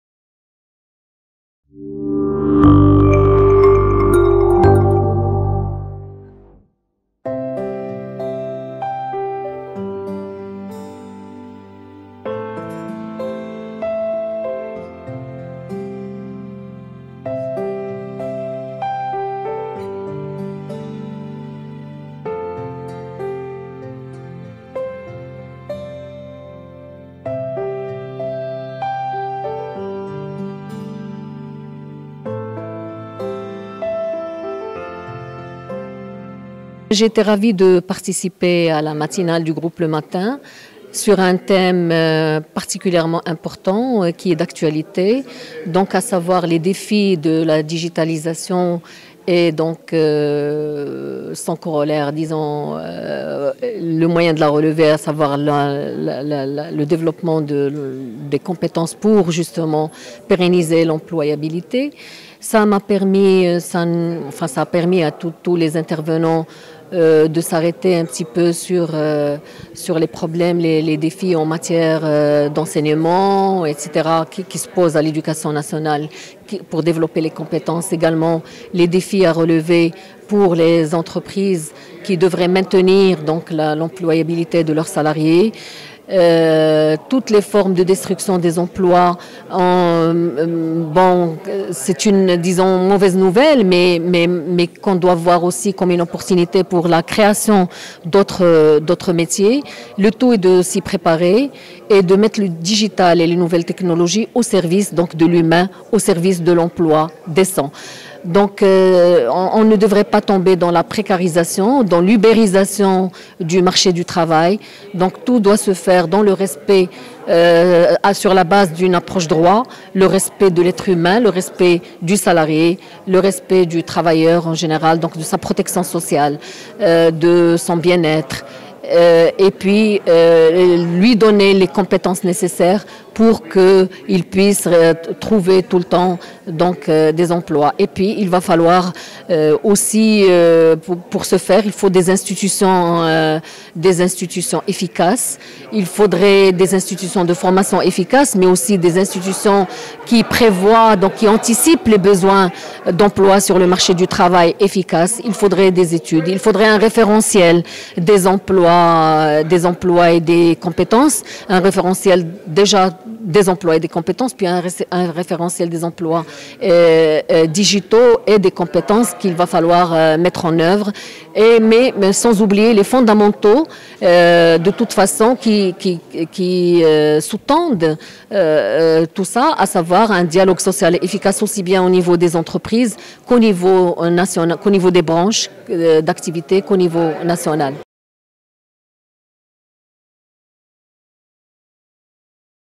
Matinale sur l'employabilité à l'ère du digital : Déclaration de Amal El Amri, SG adjointe de l'UMT
Dispositif de formation, fondamentaux sociaux, évolution des métiers, développement des compétences, upskilling, marque employeur pays, intrapreneuriat... Les intervenants de la quatrième Matinale du cycle de conférences du Groupe Le Matin ont souligné les nouveaux enjeux de l'employabilité à l'ère du digital pour une économie compétitive et pérenne.